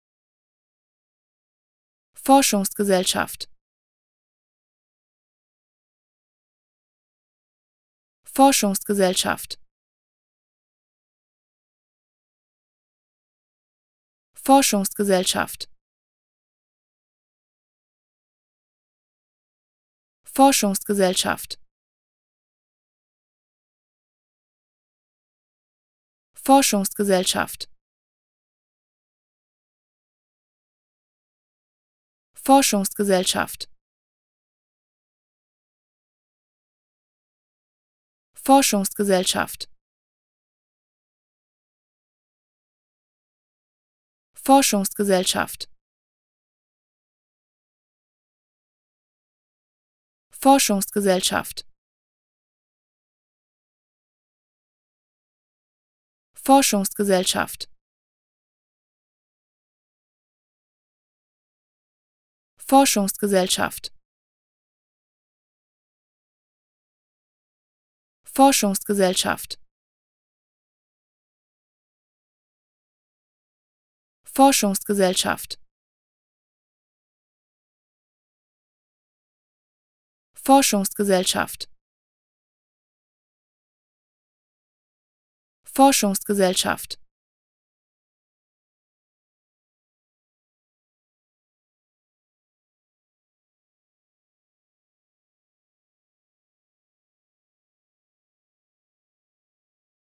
How to Pronounce ''Forschungsgesellschaft'' (Research Society) Correctly in German
Learn how to say ''Forschungsgesellschaft'' in German with this short pronunciation tutorial.